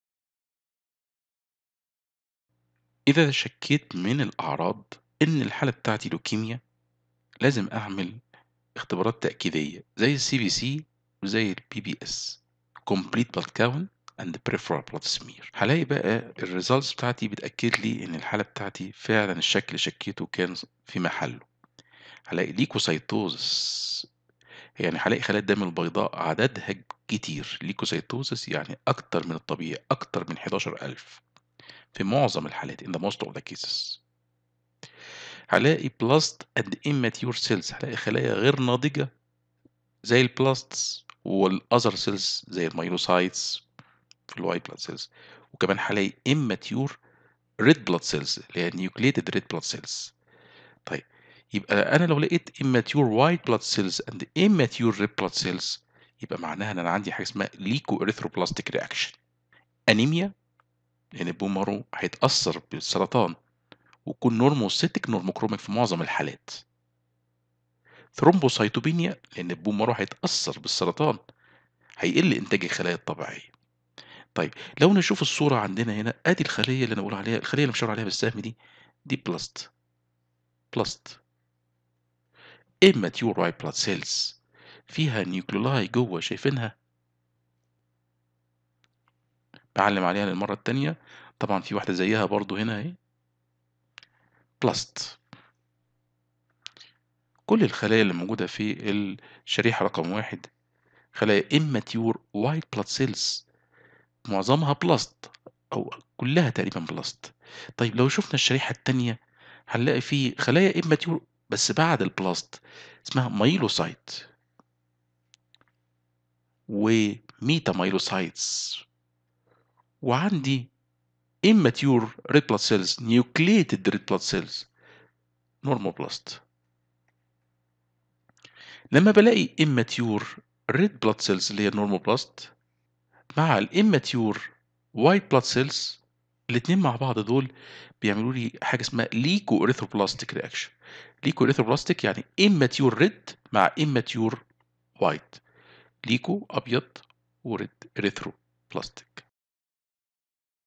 Hematology - 2 | U2-L17 | Screening diagnosis of acute leukaemias
Unit - 2 : Acute leukaemias
Lecture - 17 : Screening diagnosis of acute leukaemias
1- CBC
2- PBS
3- BM